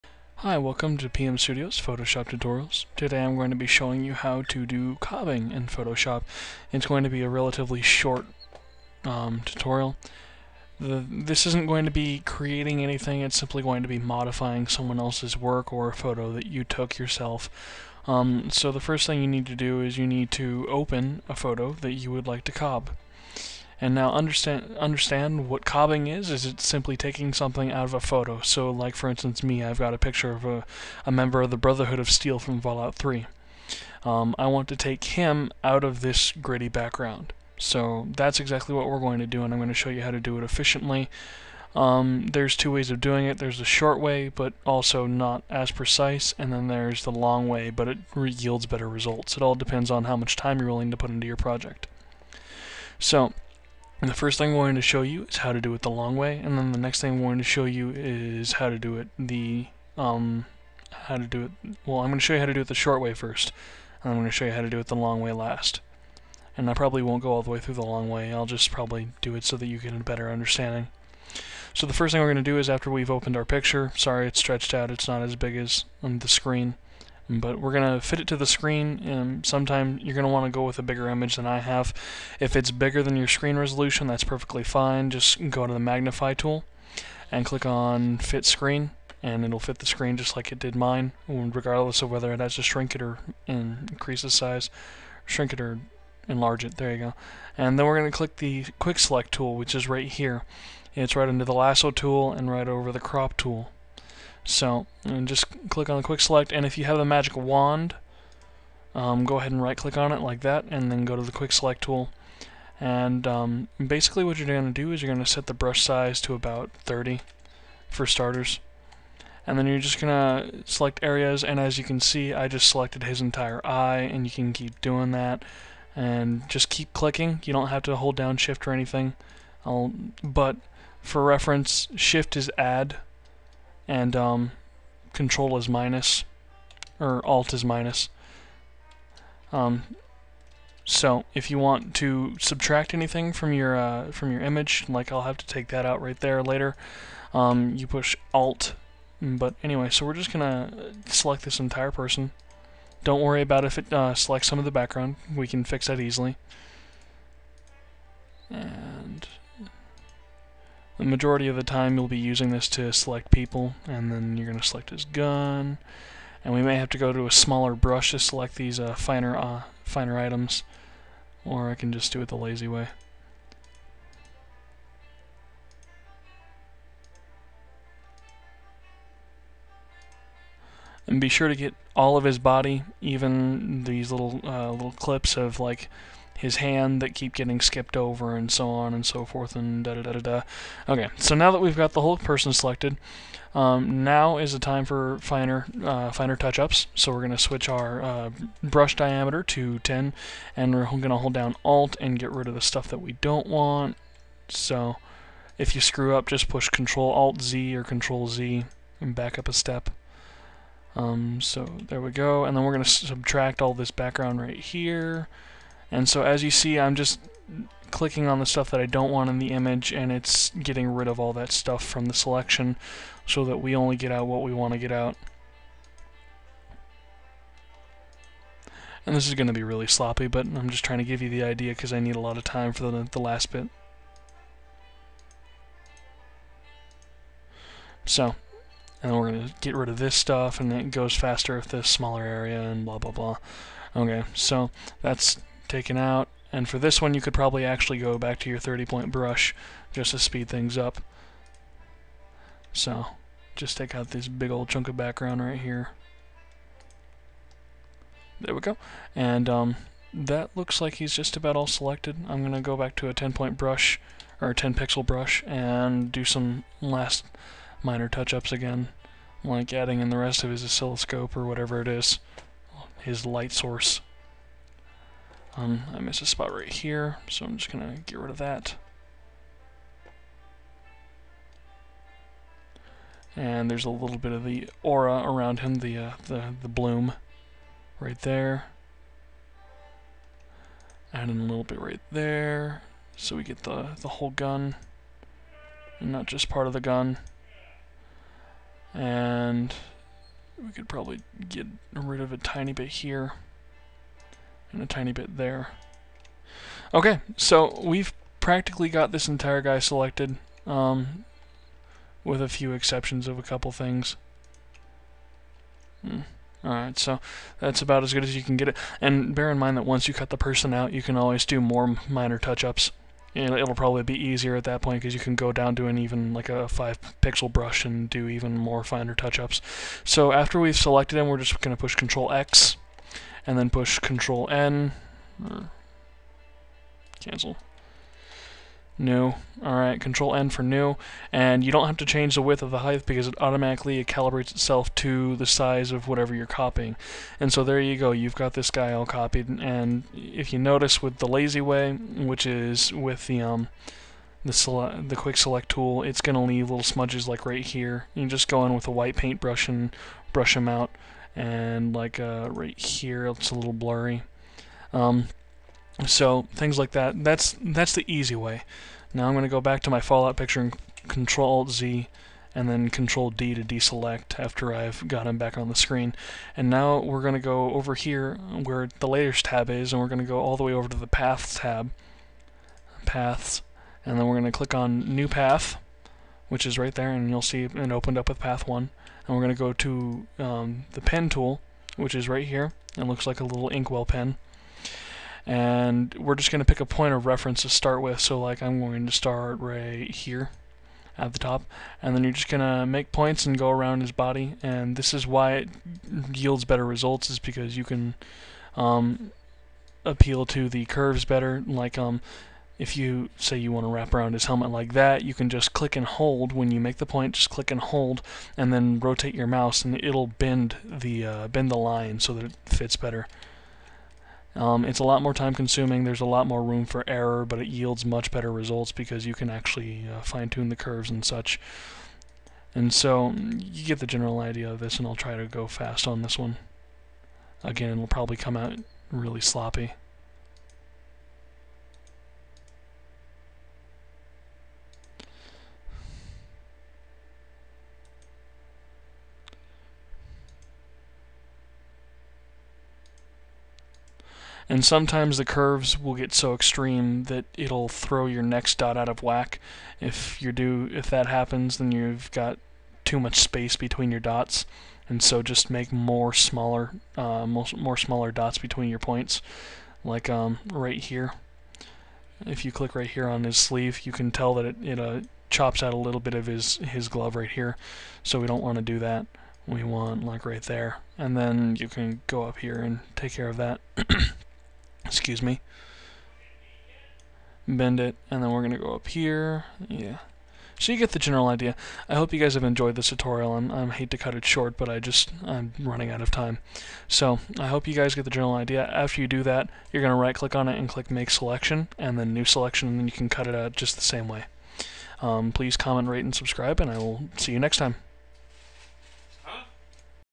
How to cobb photos in Photoshop CS4
Today i will be teaching you a more practical aspect of photoshop called cobbing.